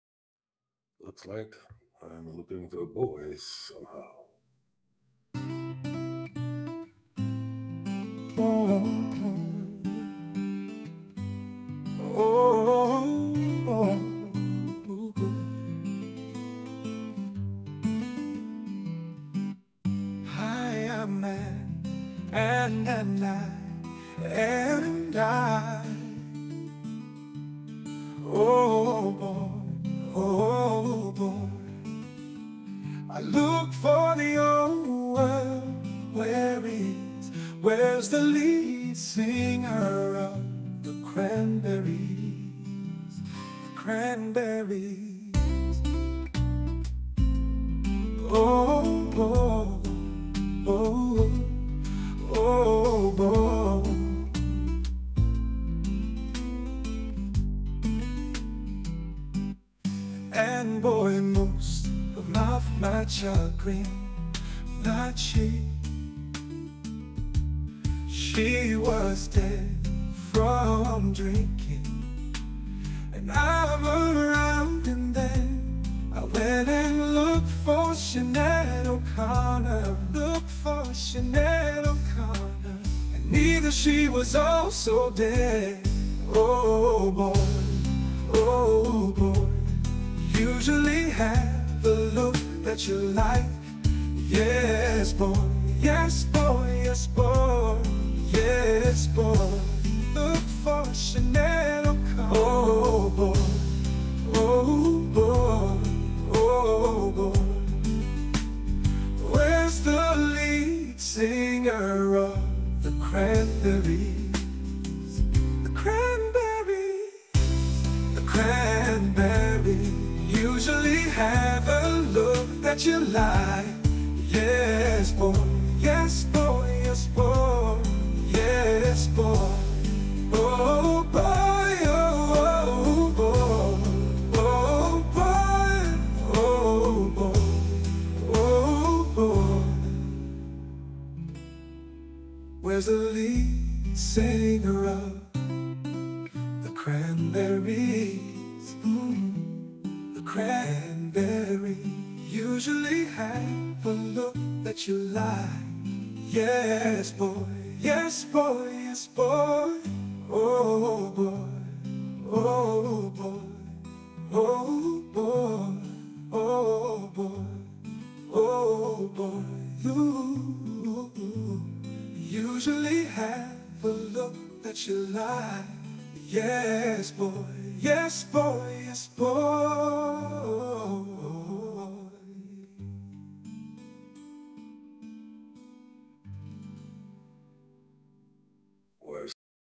oh oh boy oh oh ooo boy
hi michael and i'm really sorry for this sad sad story well o boy o boy hi michael and then o boy i looked for the singer of grandberries and o boy o boy those of mar with green she was dead from thinking i around there i went to look for her she at the corner and look for her and she was also dead always from thinking oh boy oh boy oh boy oh boy usually have to look for this you like yes boy yes boy looks like i'm looking for a boy somehow

hi i'm and  and i and i
i looked for the ore where is
where's the lead singer of
The Cranberries
(The Cranberries)

and boy most of my chagrin
not she, she was dead from drinking
and I'm around and then
I went and looked for Sinead O'Connor
(looked for Sinead O'Connor)
and neither she was also dead

usually have a look that you like
yes boy yes boy yes boy

The Cranberries
(The Cranberries)